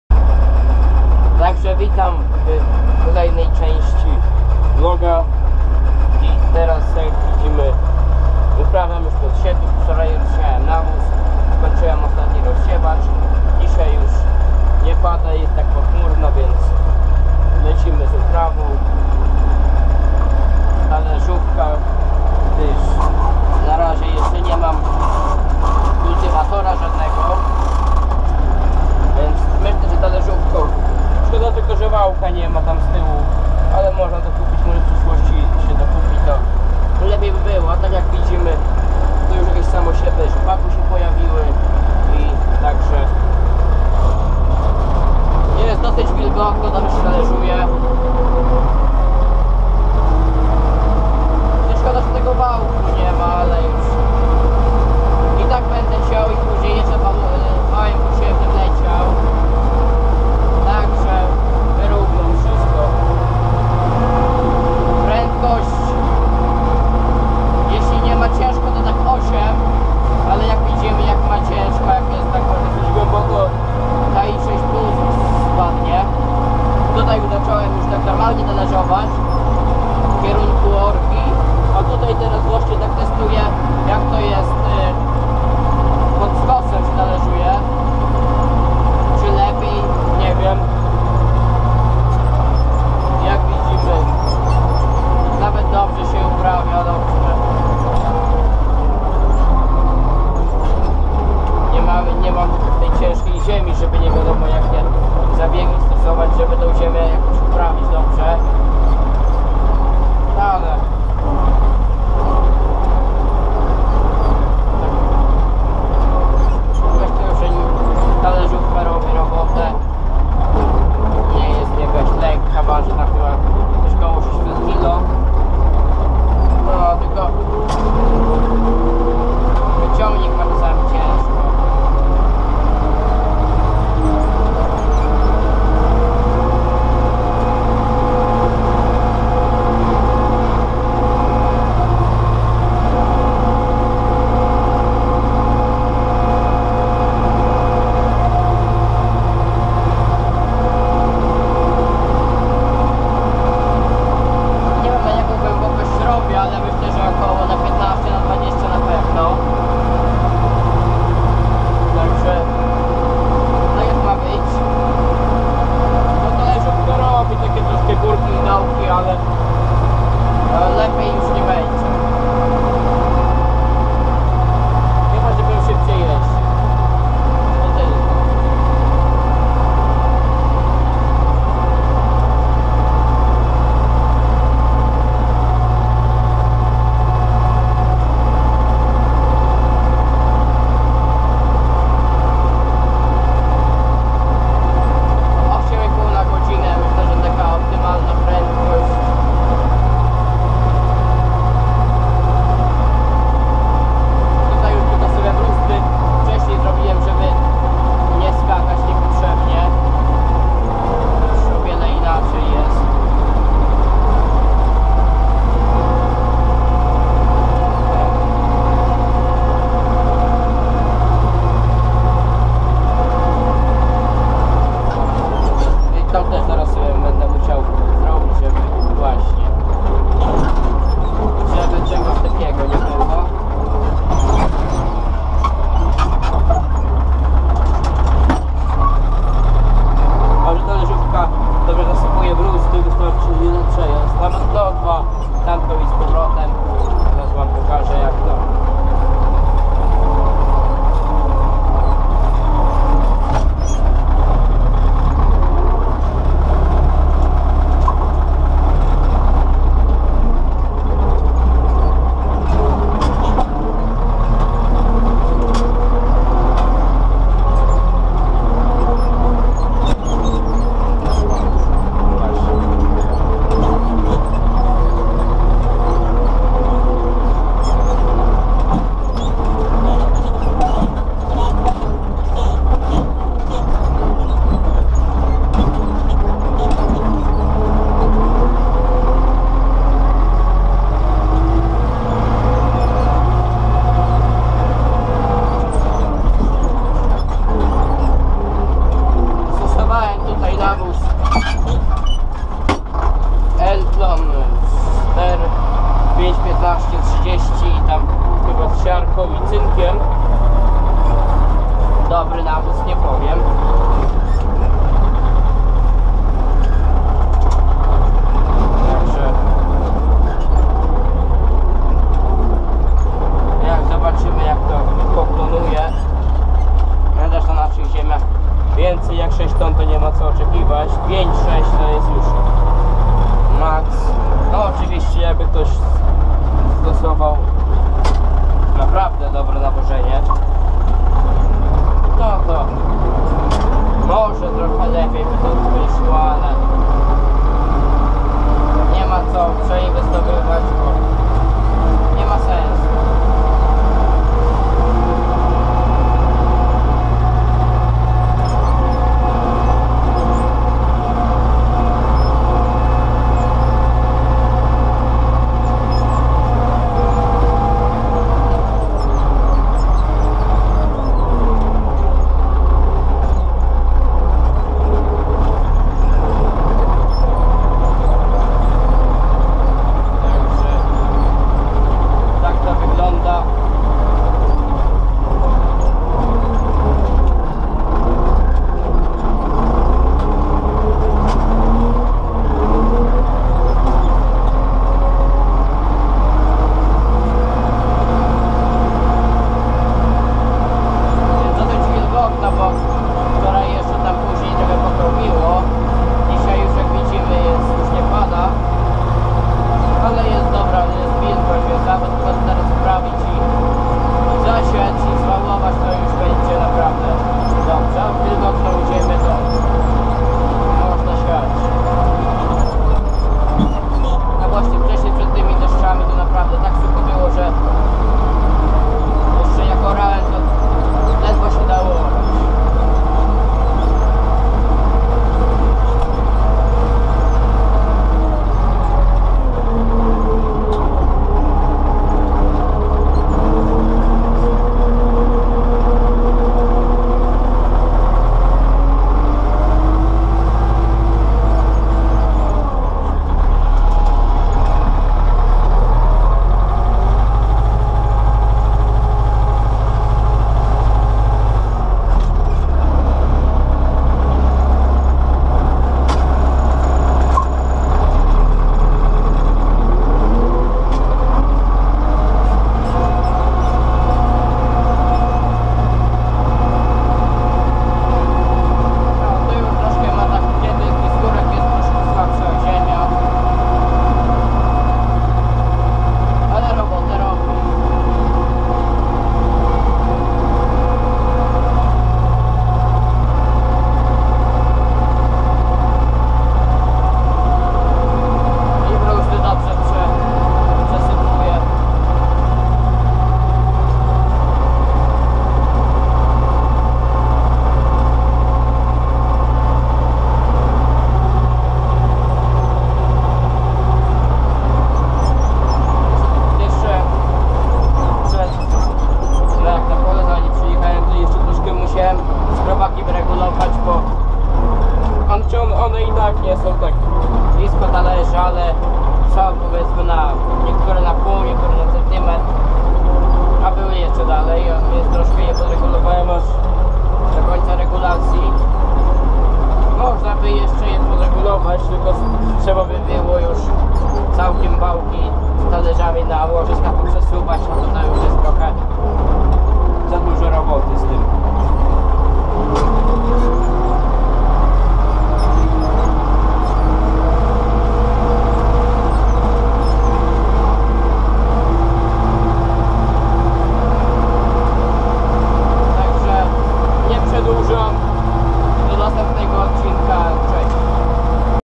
#11 Mokre talerzowanie czyli uprawa przedsiewna | MTZ & Strumyk
Witam w kolejnym vlogu, tym razem szykujemy pole pod siew, miłego oglądania :P